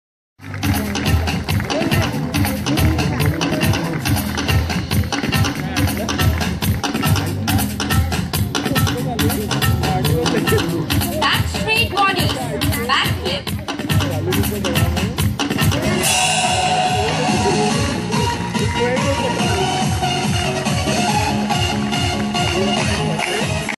Performing Acrobatic at Miramar Beach
For 127th Durand Cup, on Miramar Beach this performance was done.